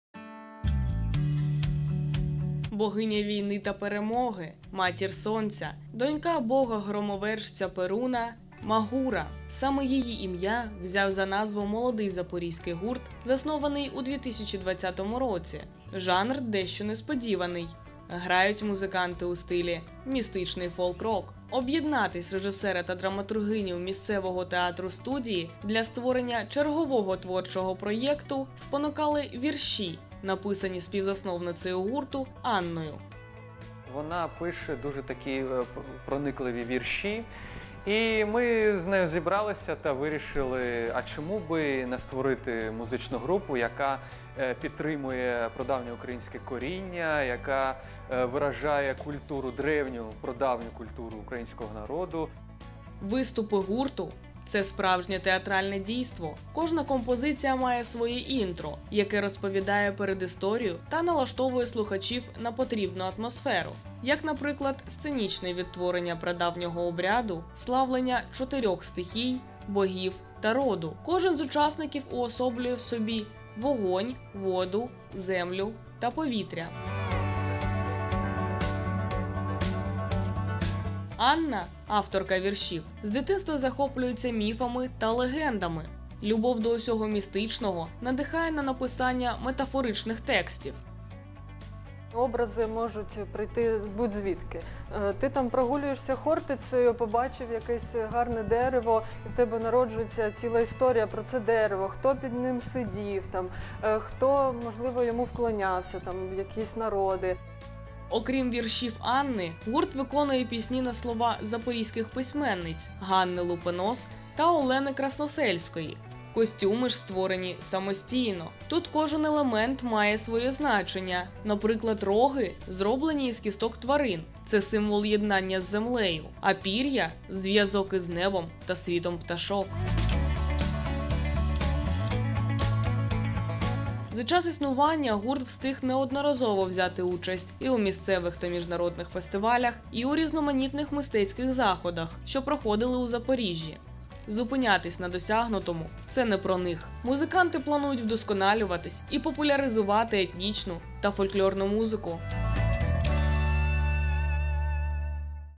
Музичний гурт “Магура” | Ранок на Суспільному
У Запоріжжі  музичний гурт “Магура” взяв за мету не лише відтворити український музичний фольклор в його автентичній манері, а й донести до глядача принципово нове прочитання народної спадщини.  Кожен їх виступ це своєрідний перформанс. Один із факторів, що зблизив учасників – любов до міфології та етнічного коріння. Навіть назва гурту має міфологічне коріння.